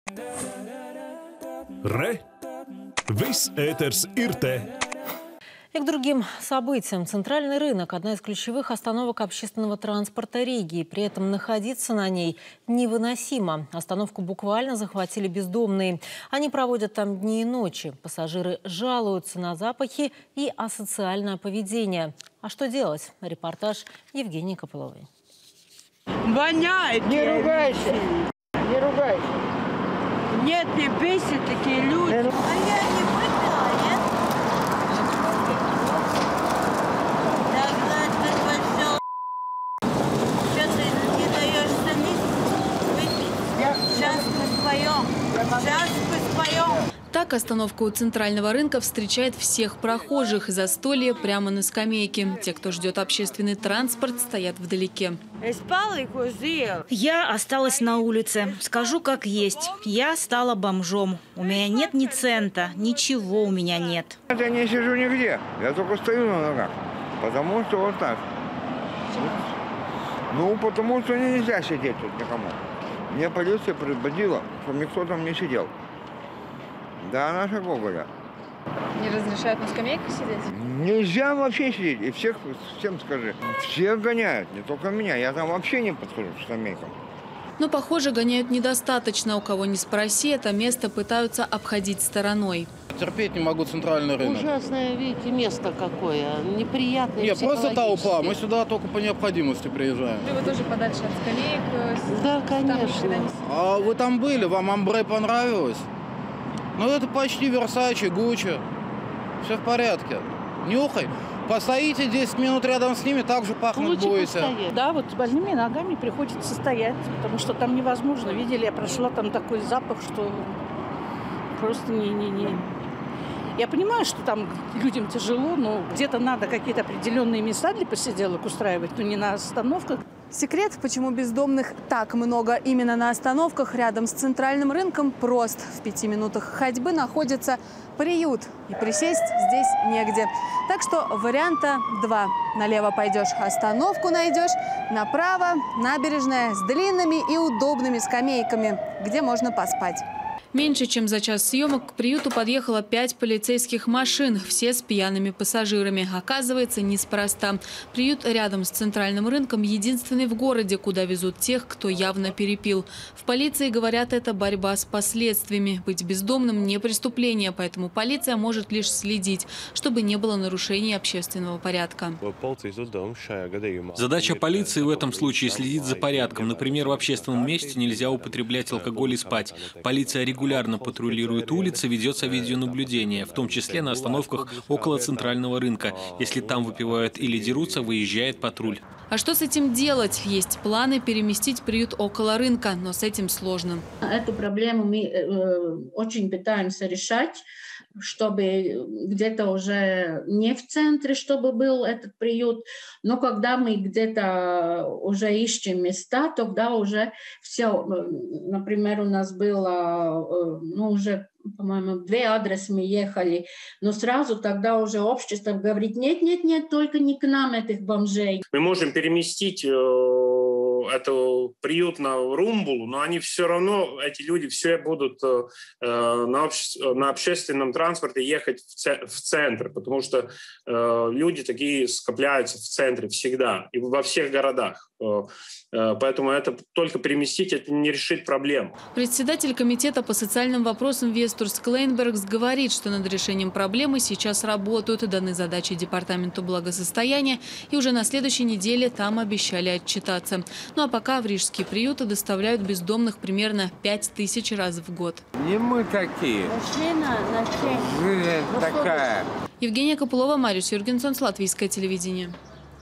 Остановка без требований
Центральный рынок — одна из ключевых остановок общественного транспорта Риги. И при этом находится на ней невыносимо. Остановку буквально захватили бездомные. Они проводят там дни и ночи. Пассажиры жалуются на запахи и ассоциальное поведение. А что делать? Репортаж Евгении Копыловой.